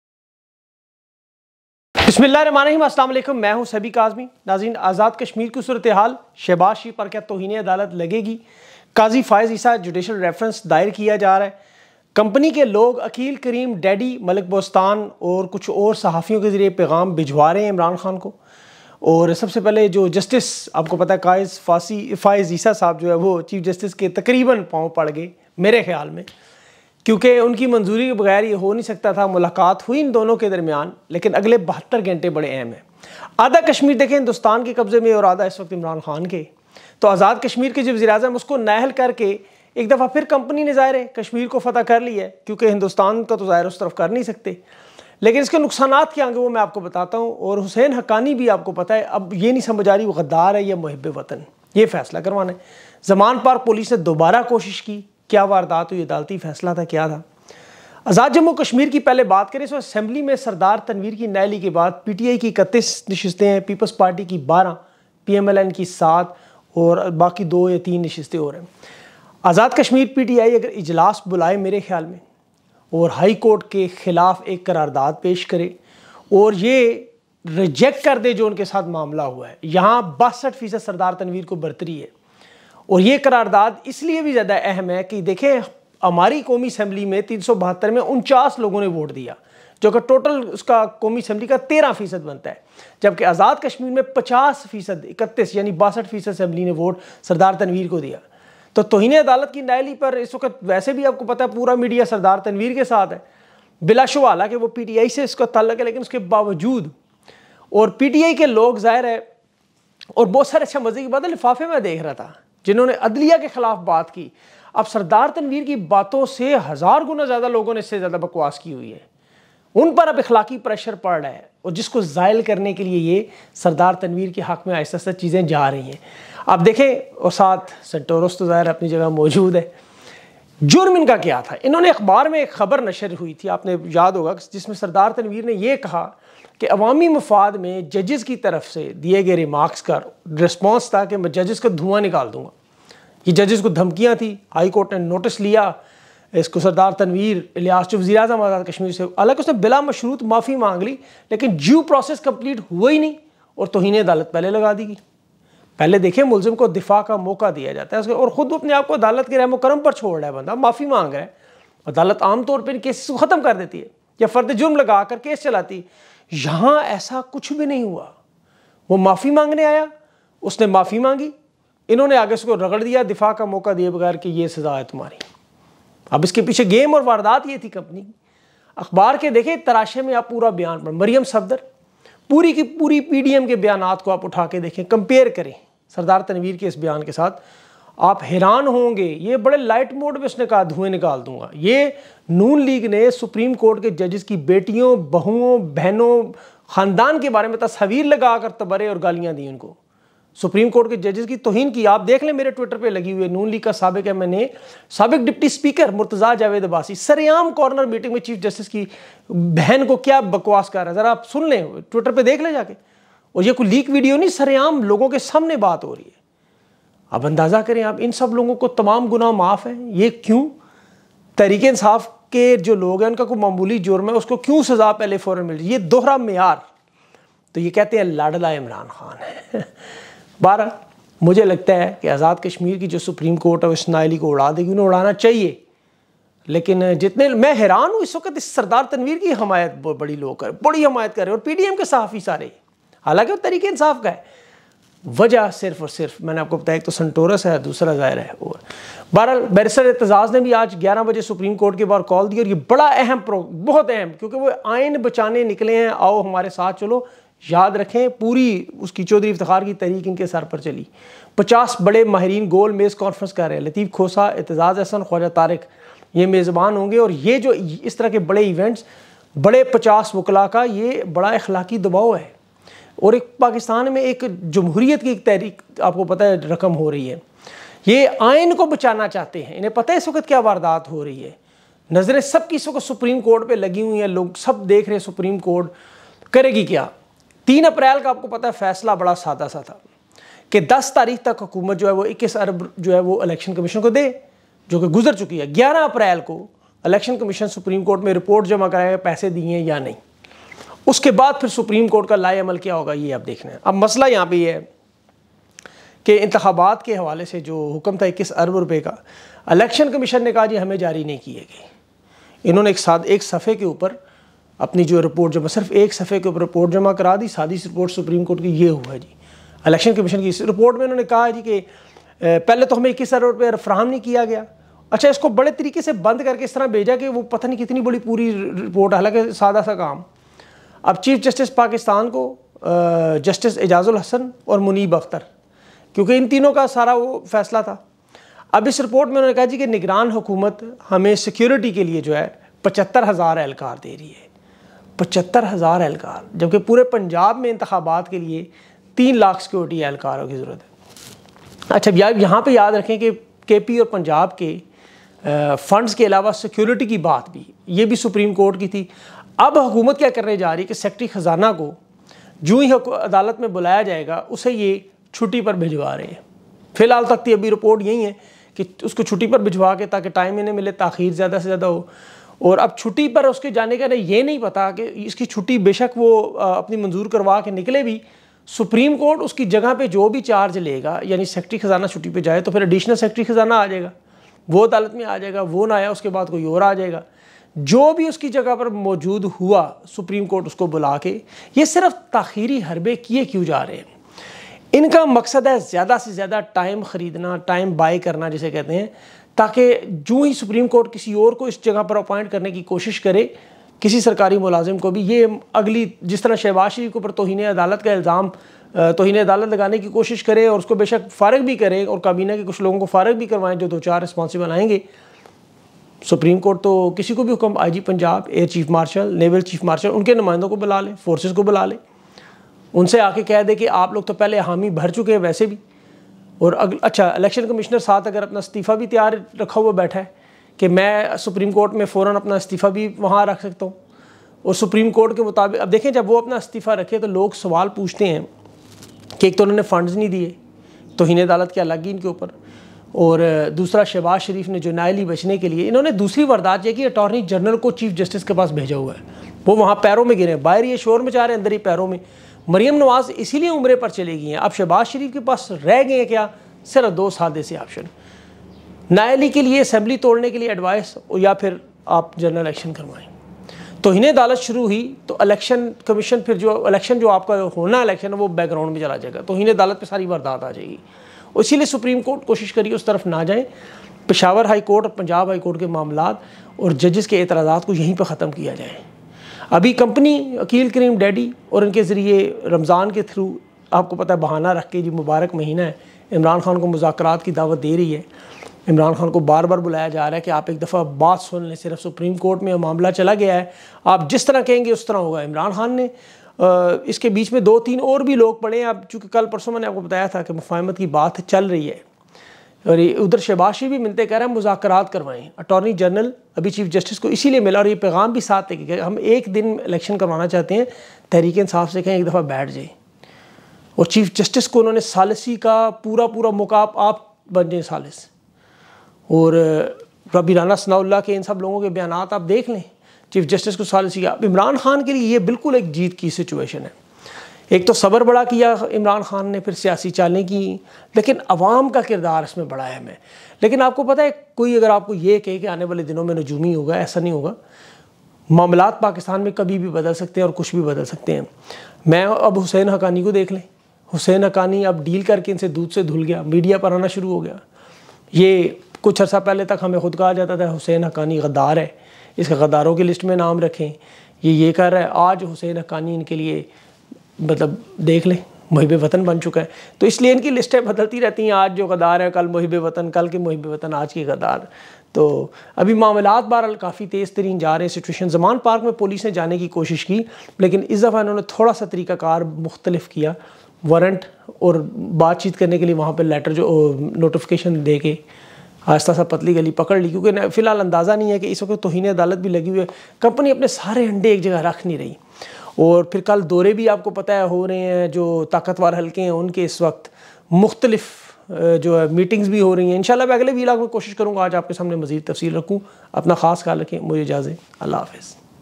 Establishment contact with Imran Khan| Supreme Court Action | Qazi Faez in trouble
Today's News Headline:
Bismillah ir Rahman ir Rahim, my name is Sabee Kazmi, and in today's video, I'll be discussing: Establishment contact with Imran Khan| Supreme Court Action | Qazi Faez in trouble
آزاد کشمری کی صورتحال، کیا شہباز شریف پر اسوقت توہین عدالت لگنے جارہی ہے،
قاضی فاہز عیسیٰ پر جیوڈیشل ریفرنس بھی داہر کیا جا رہا ہے
کمپنی کے لوگ عقیل کریم ڈھیڈی، ملک بوستان، سہیل وڑاہیچ کے زریعے کیا پیغام پجھوارہیں ہٰں
۔آنے والے 72 گھنٹے اہم ہے
حسین حقانی کو اب غدار کہنا ہے یا محب وطن یہ زرا فیصلہ کروالیں کمپنی سے
زمان پارک پولیس کی دوبارہ کوشش تھی یا عدالتی فیصلہ؟
،‏آزاد جموں کاشمیر اسمبلی۔۔ سردار تنویر کی نااہلی کے بعد
Get updates on all these latest Pakistan live news headlines; Establishment contact with Imran Khan| Supreme Court Action | Qazi Faez in trouble in today's Sabee Kazmi latest analysis!